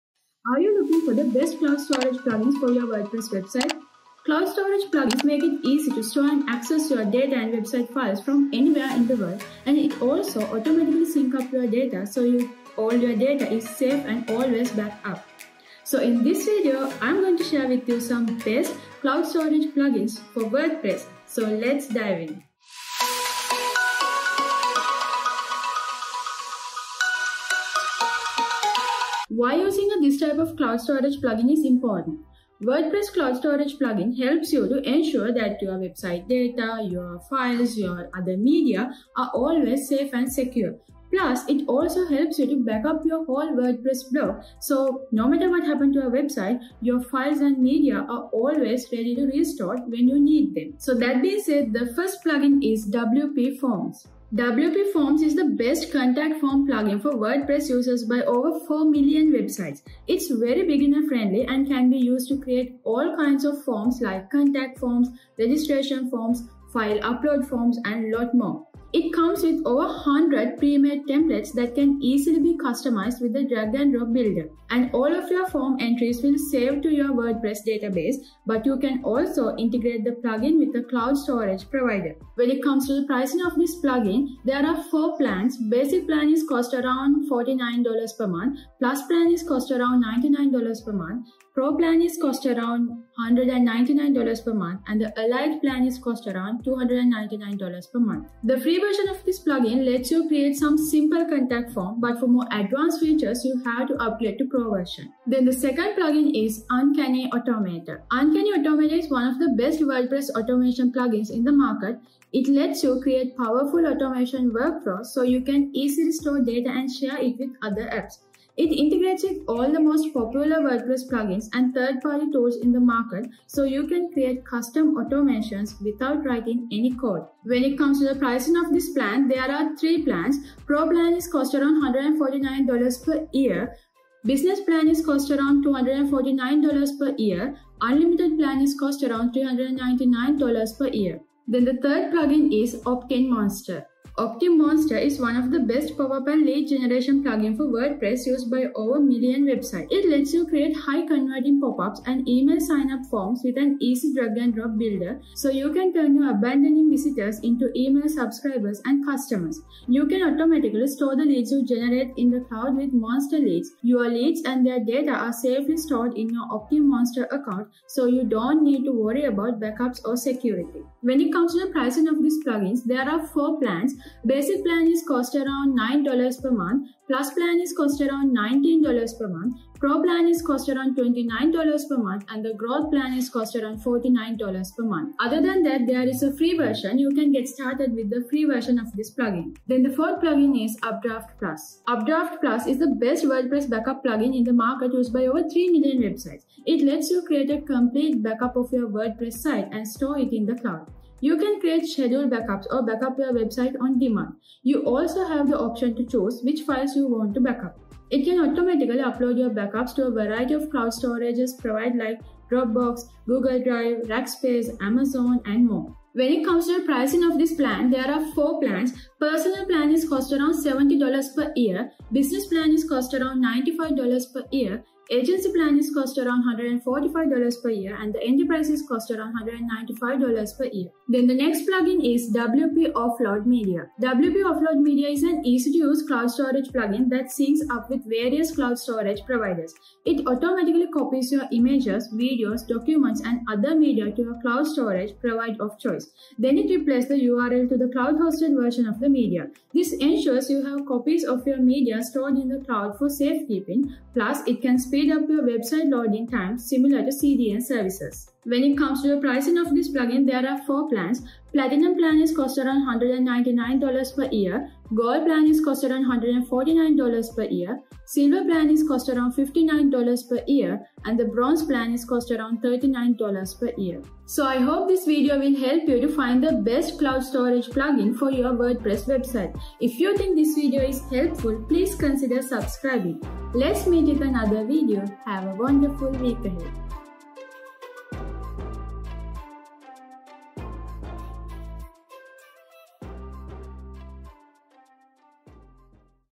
5 Best WordPress Cloud Storage Plugins 2022 (w/ Free Options)
Cloud storage plugins make it easy to store and access your website files from anywhere in the world. They can also automatically sync your data, so it’s safe and always backed up.

In this video, we’ll share some of the best WordPress cloud storage plugins you can use to store your WordPress files.